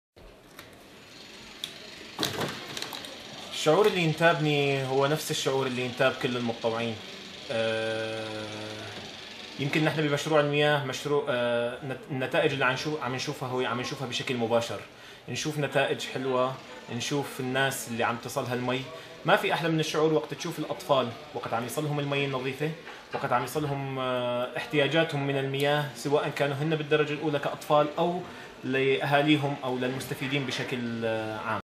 Firas, Syrian Arab Red Crescent Volunteer
Il Movimento Internazionale della Croce Rossa e Mezzaluna Rossa lancia la campagna Thunderclap per sostenere la Mezzaluna Rossa Siriana